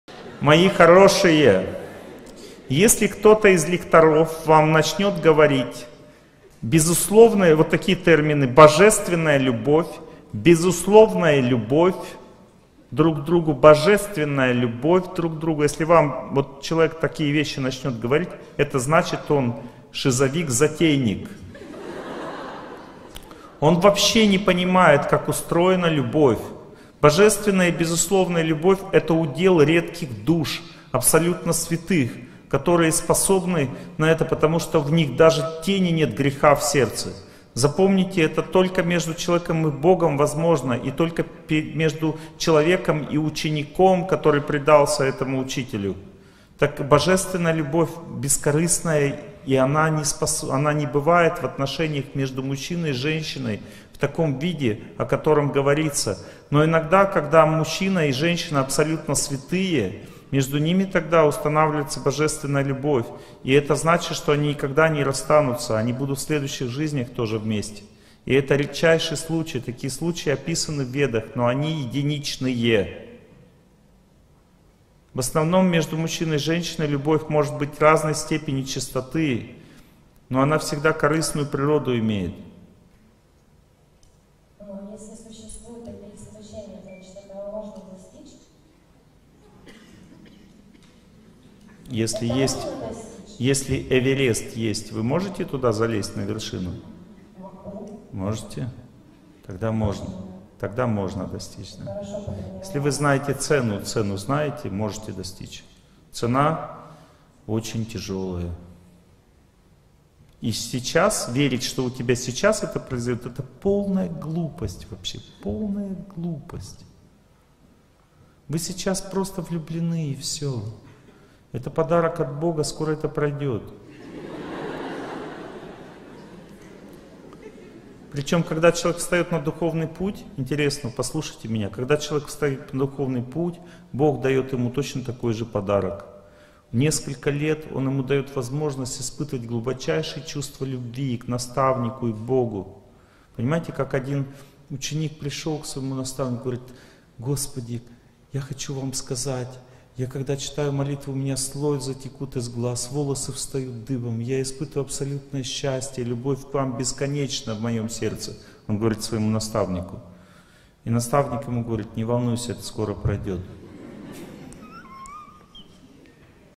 Торсунов О.Г. Между мужчиной и женщиной не бывает божественной любви
В этом видео Олег Геннадьевич Торсунов говорит о том , что между мужчиной и женщиной не бывает божественной любви
Из лекции "Воспитание хороших черт характера""  Красноярск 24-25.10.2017